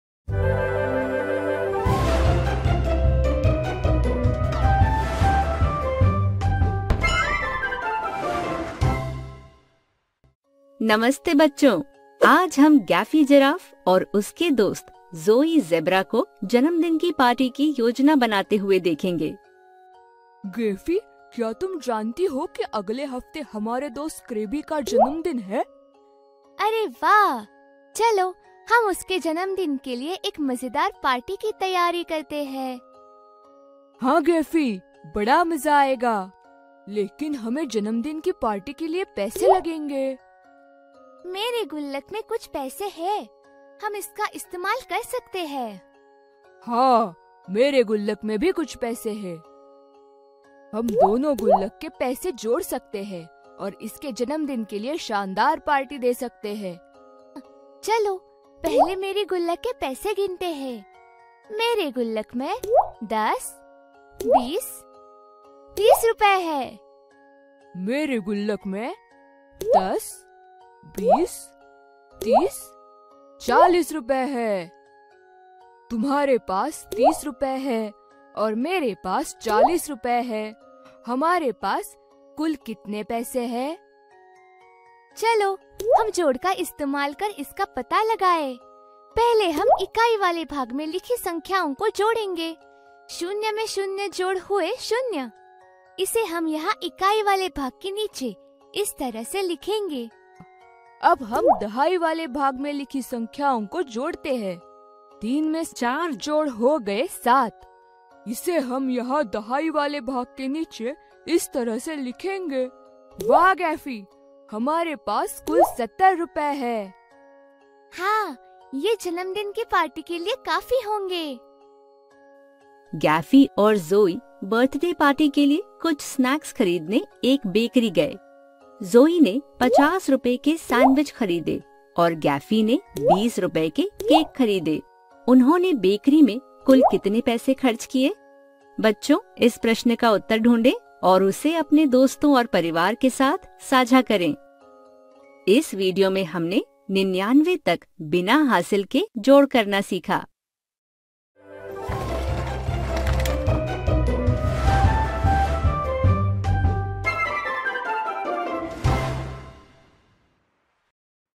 दो अंकों वाली संख्याओं को जोड़ना|Addition without carryover till 99- Part 1 |NumberSense|TicTacLearn
प्रिय विद्यार्थियो,

हैप्पी लर्निंग!

Gaffy
Nursery
Foundational Numeracy
NIPUN
Fun with Numbers
FLN
Adds 2-digit numbers in vertical/horizontal algorithm without regrouping